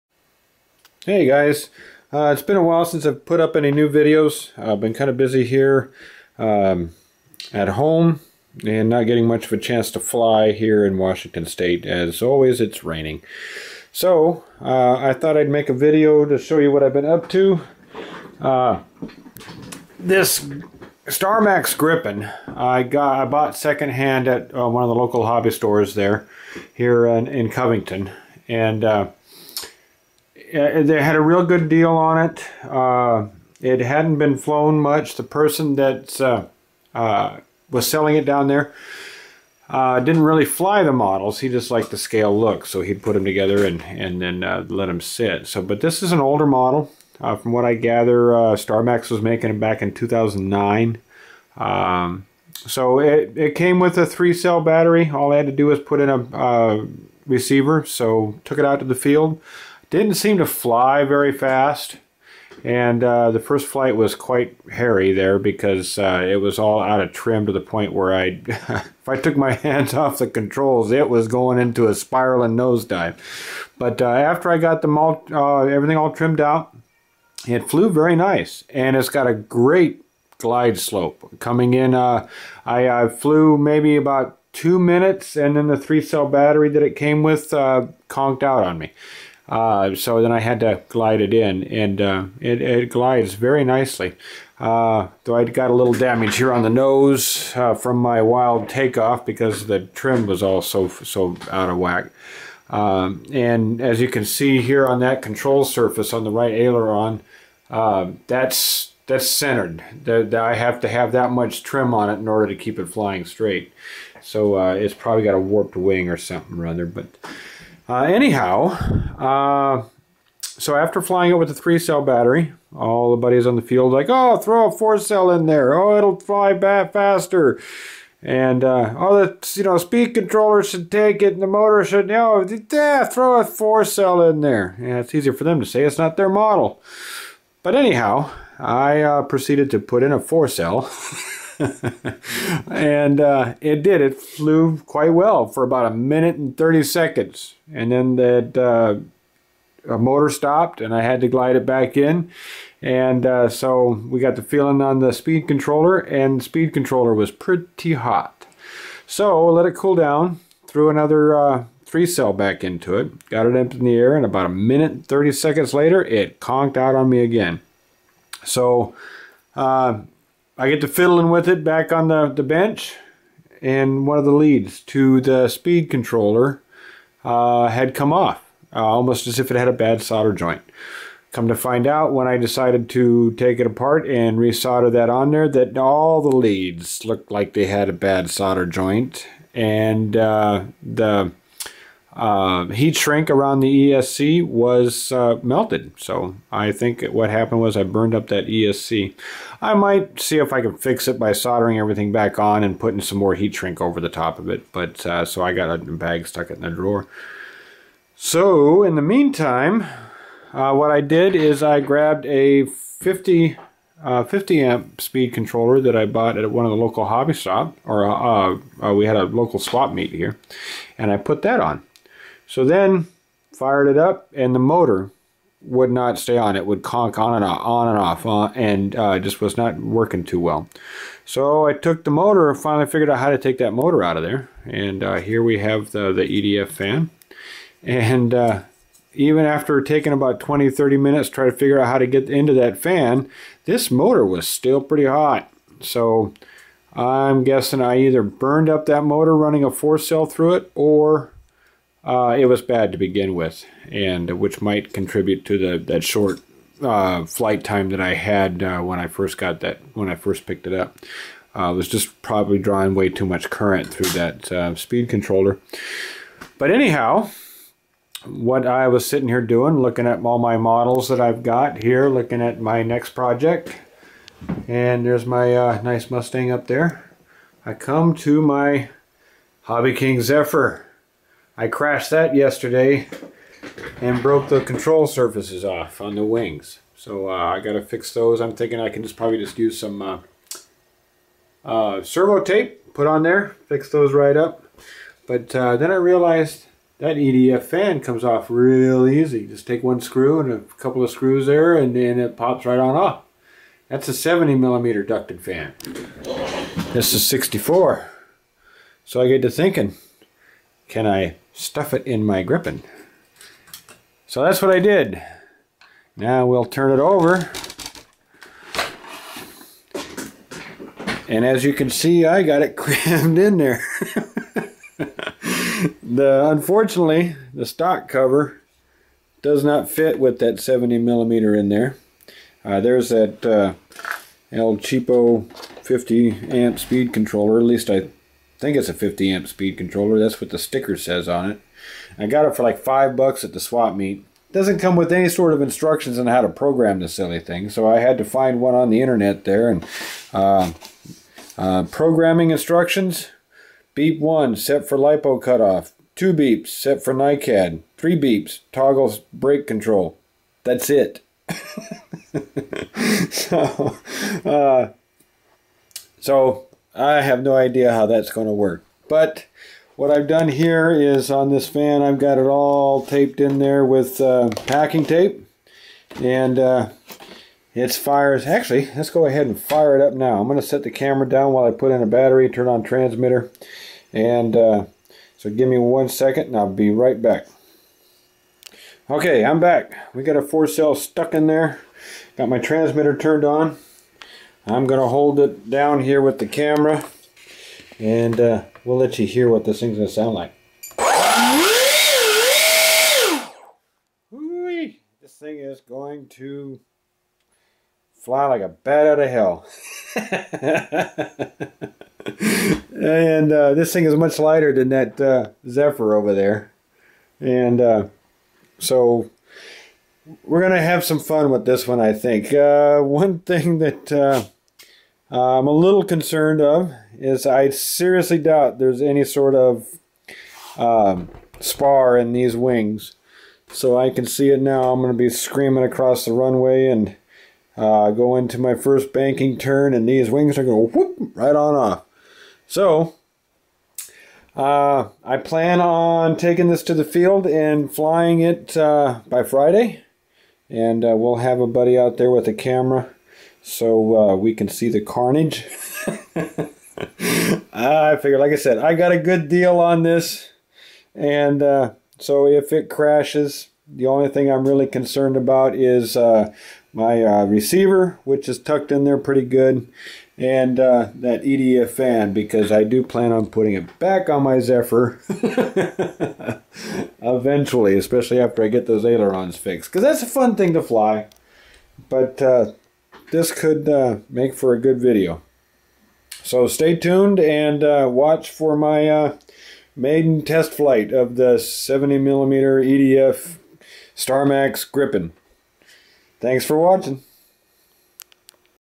Starmax Gripen 70mm EDF Modification
I was looking for something to do, and come up with the idea of taking the 70mm EDF fan out of my Hobby King Zephyr, and putting it into my 64mm EDF Starmax Gripen!  I hope to have a maiden flight of this plane and it's EDF modification here in the next couple of days, with a the video up by this weekend. =)